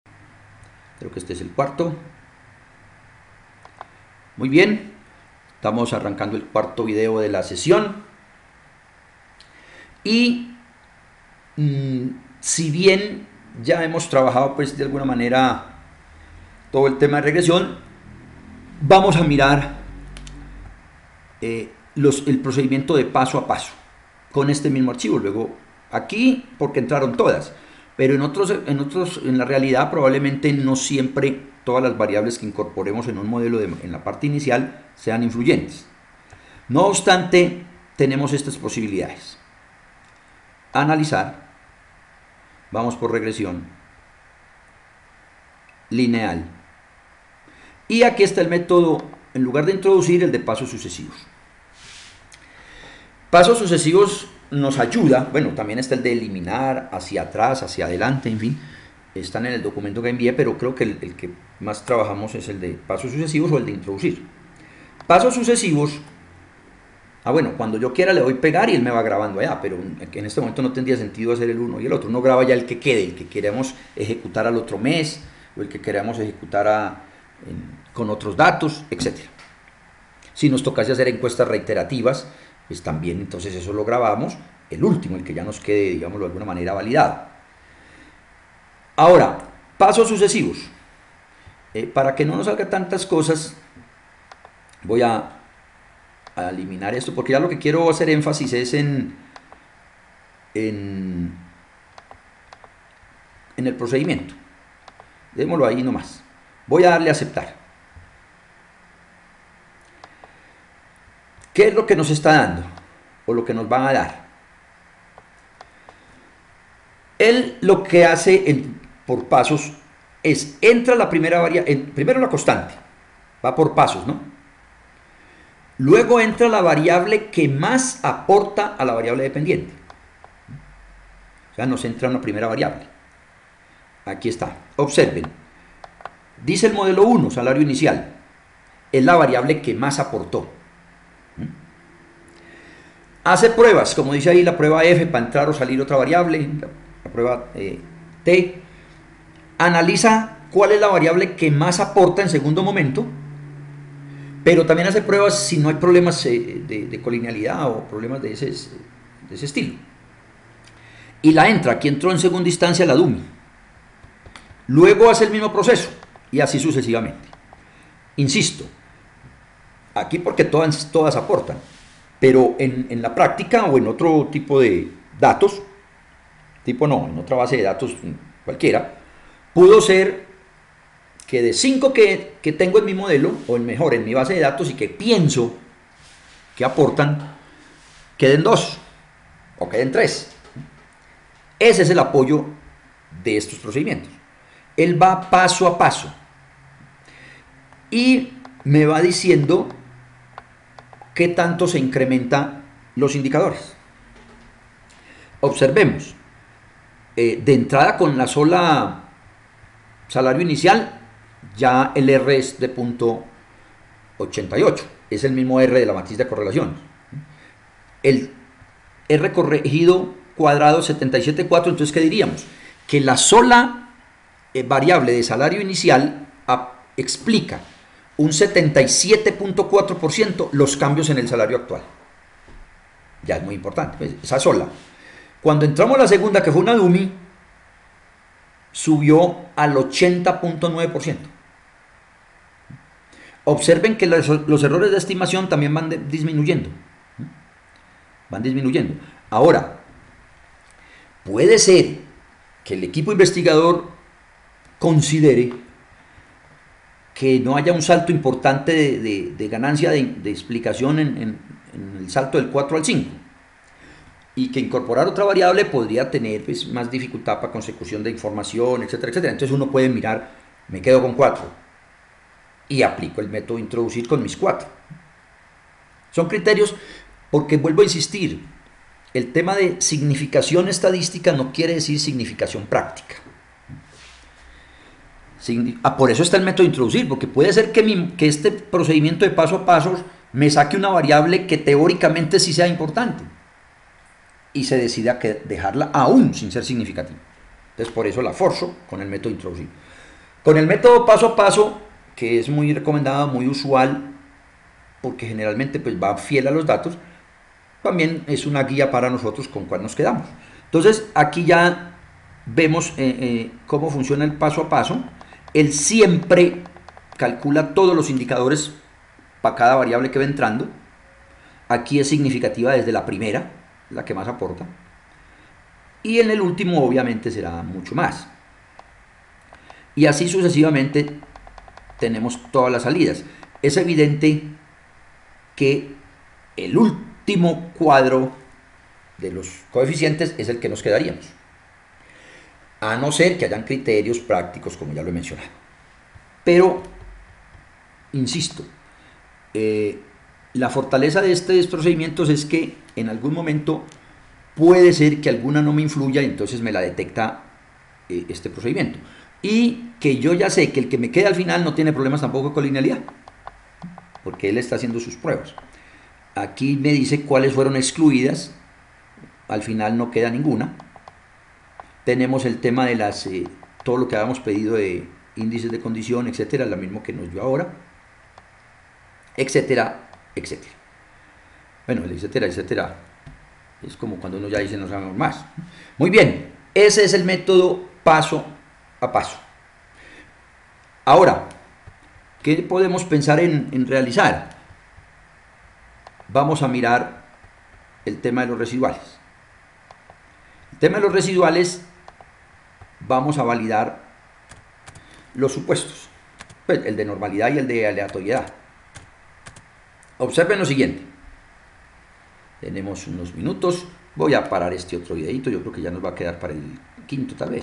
Regresión paso a paso SPSS
Regresión paso a paso  con SPSS, diagnóstico de colinealidad, indice de condicionamiento, curso online, econométria, León Darío Bello